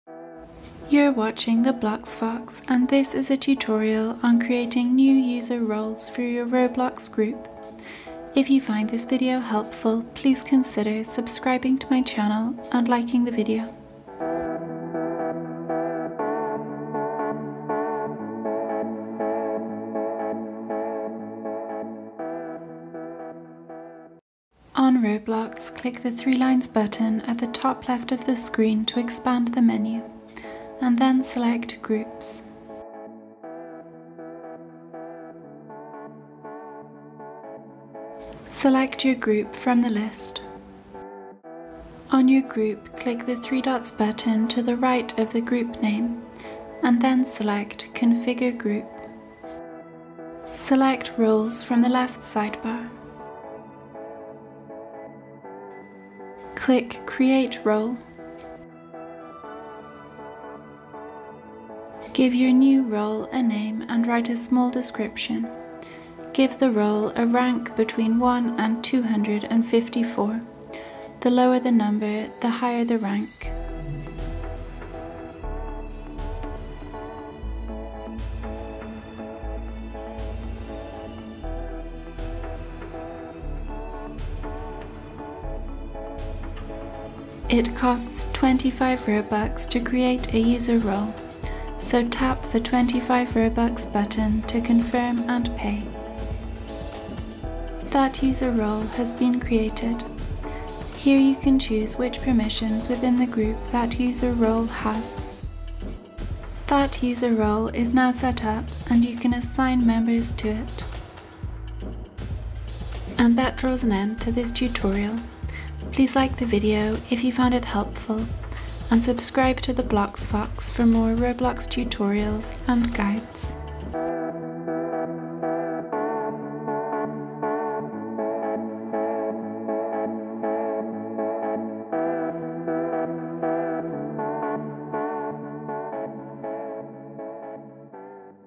How To Create User Roles For Your Roblox Group
Tutorial on creating new user roles for your Roblox group. Want to make your own user roles for your Roblox group? It’s easy to do and you can do it both on desktop and mobile. Learn how to set up new user roles on Roblox now!

Steps:

On Roblox, Click the 3 lines button at the top left of the screen to expand the menu and then select ‘groups’.

Select your group from the list.

On your group, click the 3 dots button to the right of the group name and then select ‘configure group’.

Select ‘roles’ from the left sidebar.

Click ‘create role’.

Give your new role a name and write a small description. Give the role a rank between 1 and 254, the lower the number the higher the rank.

It costs 25 Robux to create a user role, so tap the 25 Robux button to confirm and pay.

That user role has been created, here you can choose which permissions within the group that user role has.

That user role is now set up and you can assign members to it.